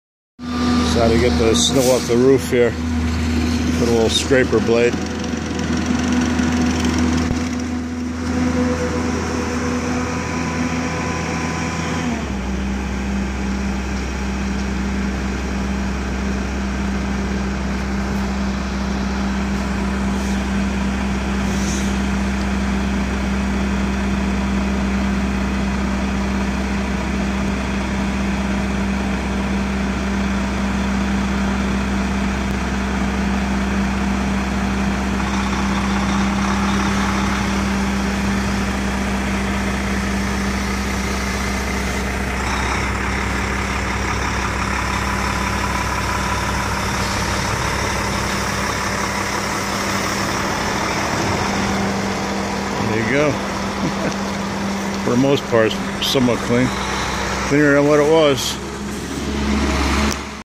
Shoveling snow off semi trailer the easy way
Remove snow off Semi Truck